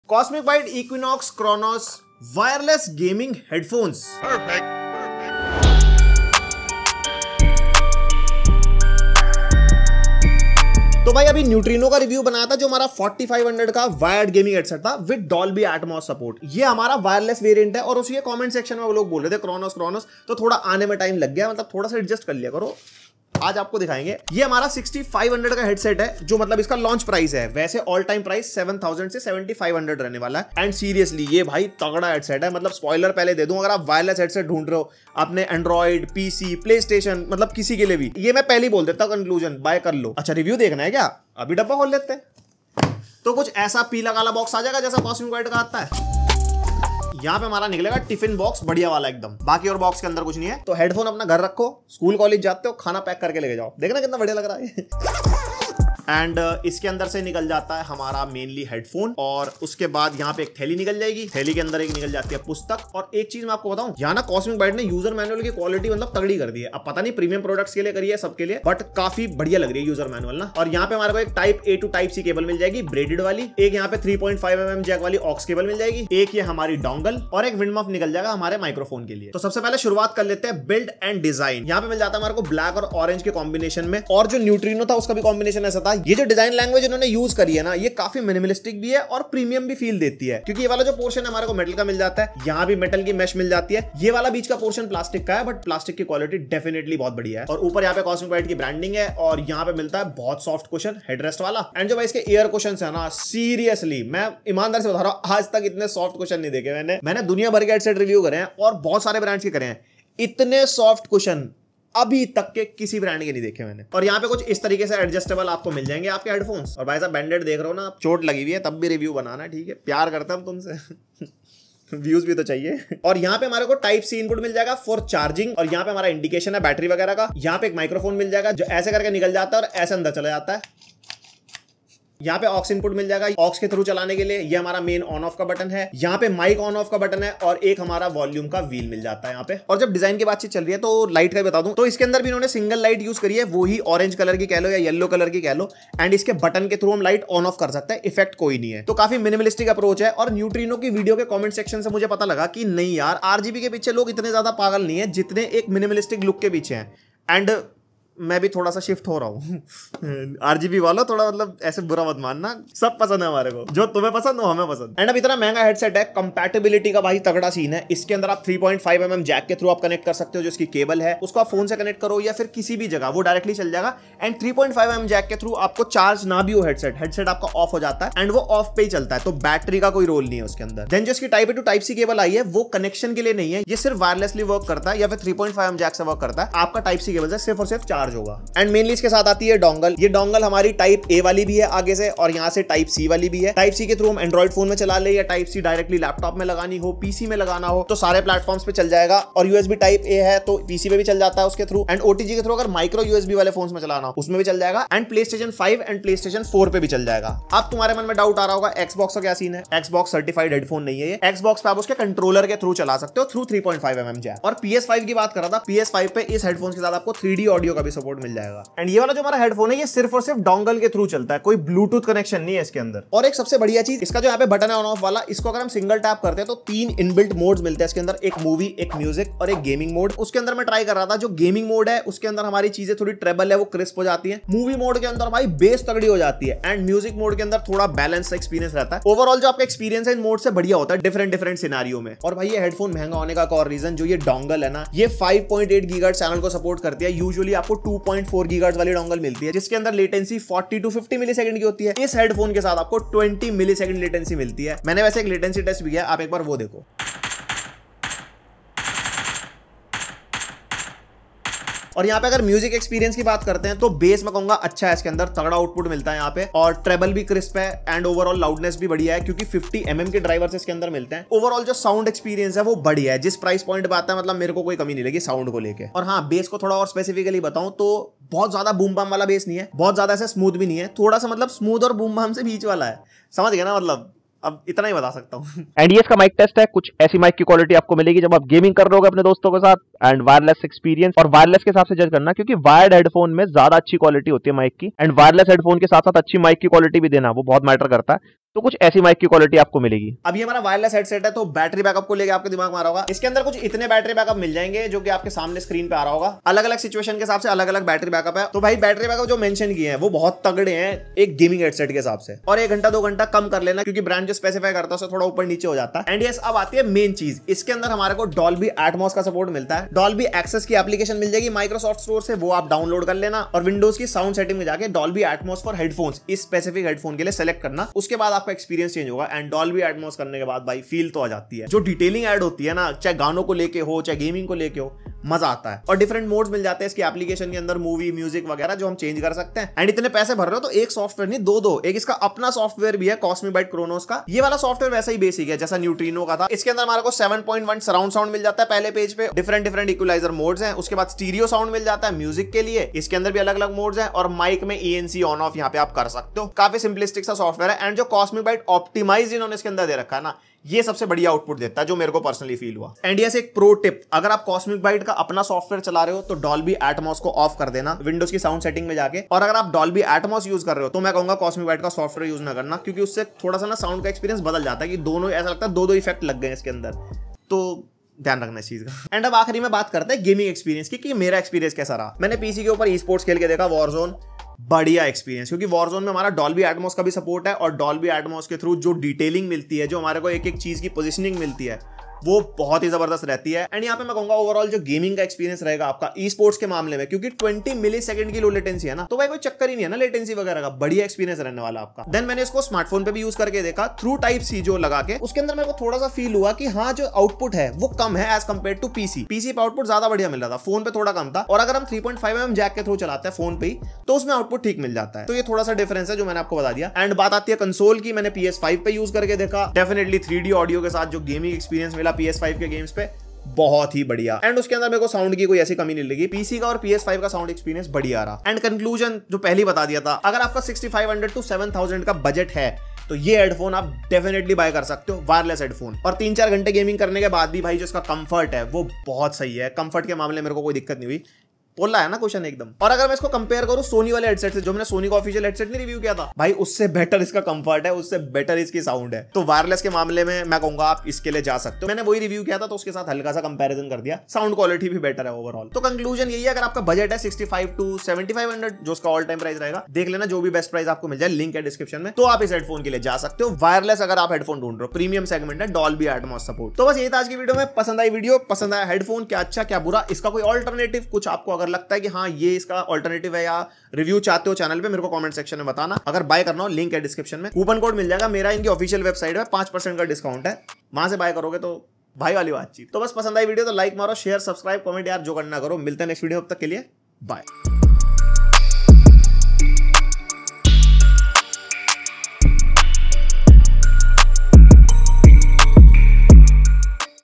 Wireless Gaming Headphones With Dolby Atmos | Cosmic Byte Equinox Kronos
Cosmic Byte Equinox Kronos Wireless 5.8Ghz Gaming Headset, Dolby Atmos, Extreme Low Latency 20ms, 7.1 Surround Sound

My Gears:-

Primary Camera

Secondary Camera